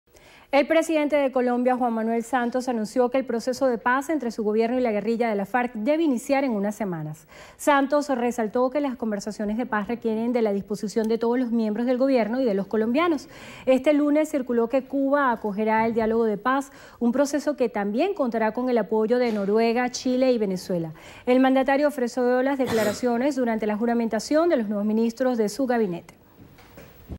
Santos anuncia inicio de proceso de paz tras establecer hoja de ruta con FARC
El presidente de Colombia, Juan Manuel Santos, anunció el inicio de un proceso de paz con las FARC tras confirmar que han concluido las "conversaciones exploratorias" con la guerrilla y lograr un acuerdo marco "que establece una hoja de ruta".